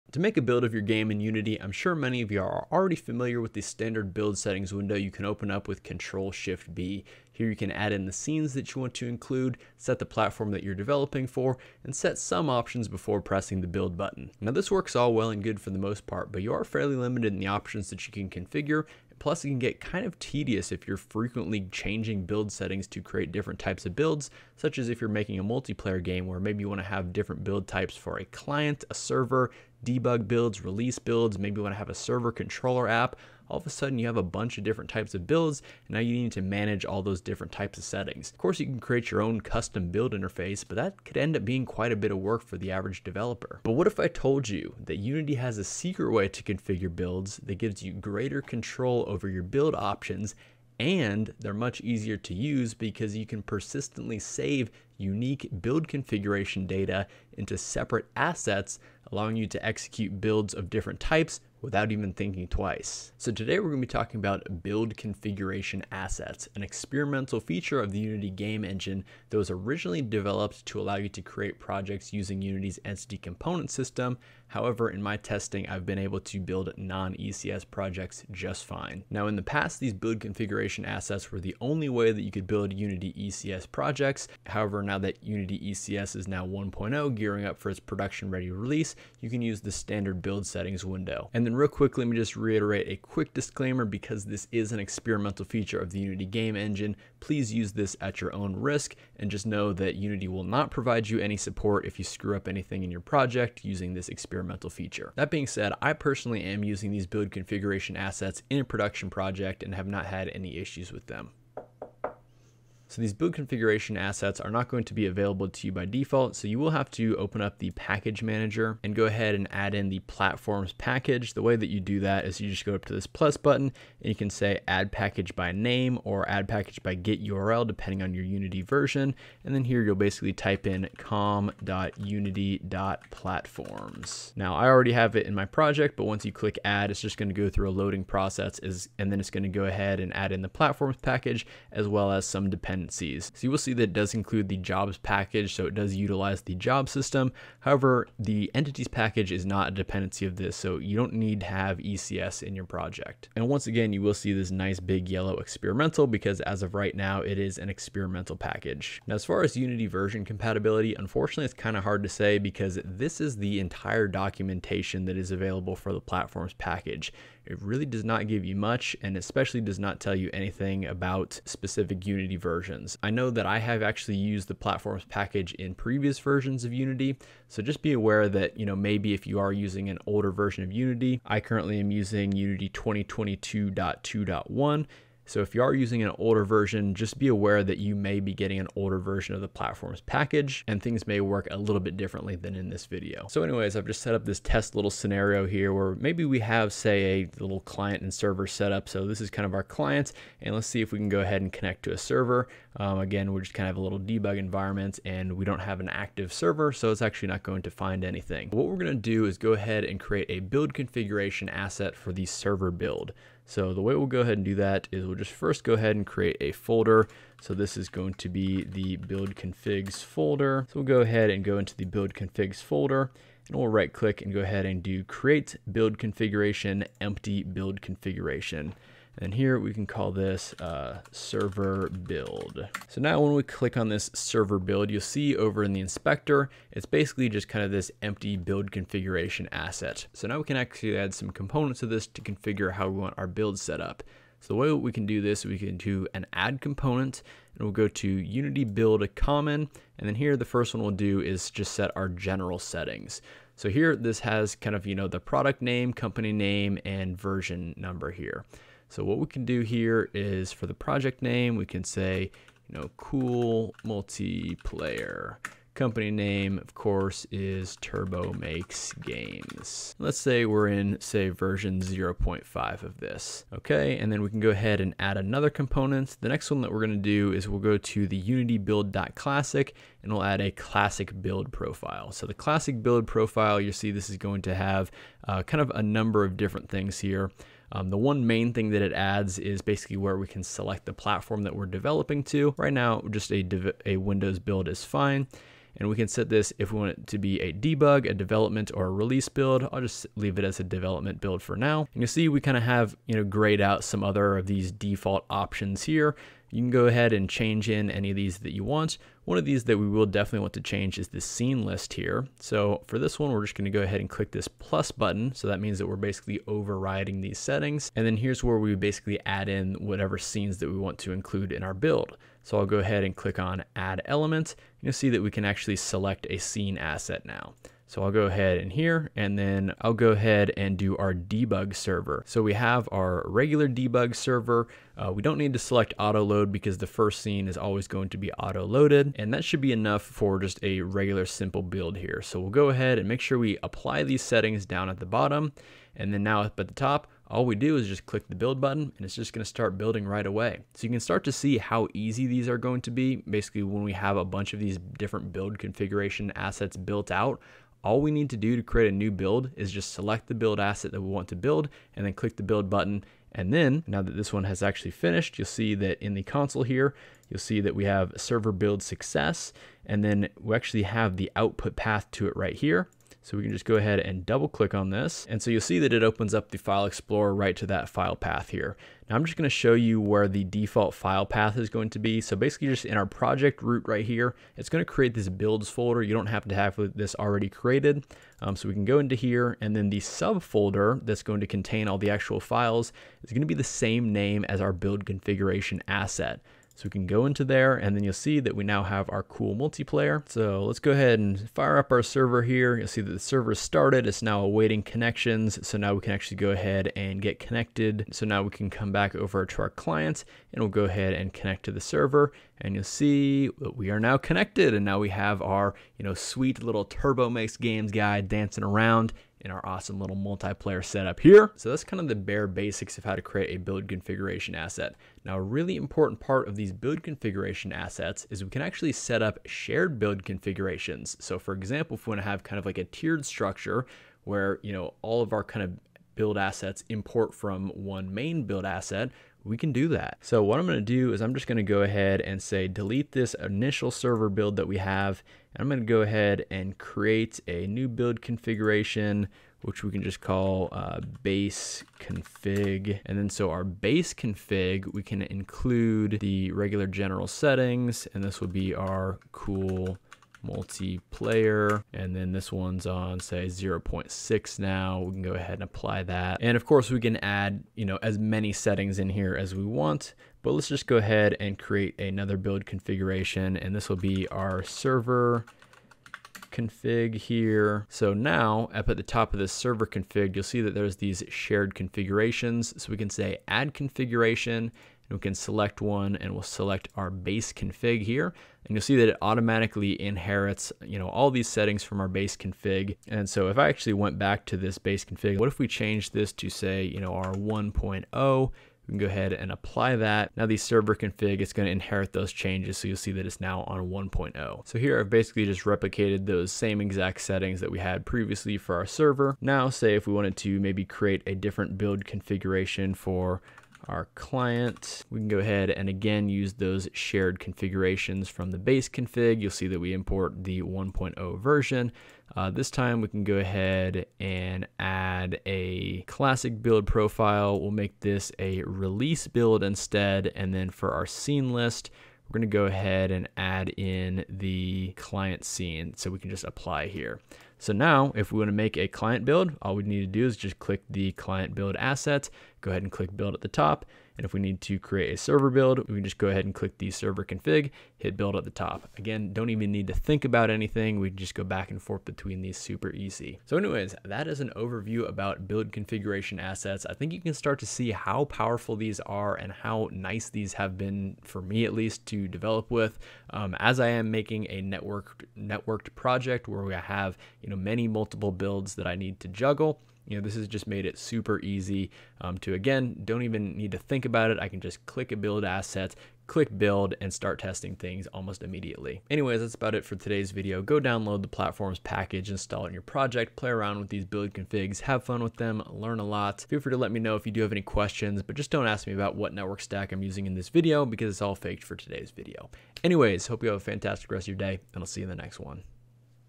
STOP Building Unity Games Like This - [Build Configs Tutorial 2023]
▶ Platforms Package: com.unity.platforms ◀

🚧 Resources Mentioned 🚧

🎮 Let me know what other topics you want to learn about 🎮

⌚ Time stamps for key topics ⌚

- 0:00 - Build Settings Window
- 0:43 - Build Configuration Assets
- 1:52 - Installing Platforms Package
- 3:13 - Creating a Build Config Asset
- 5:39 - Building from Config Assets
- 7:01 - Shared Build Configs (Inheritance)
- 9:13 - Final Thoughts on Build Configs